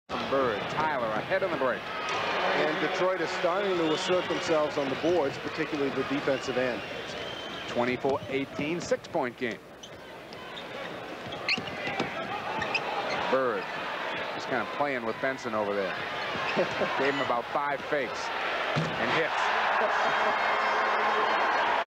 Larry Bird embarrassing a former number 1 draft pick
Larry Bird faking out former 1977 NBA number one overall draft pick Kent Benson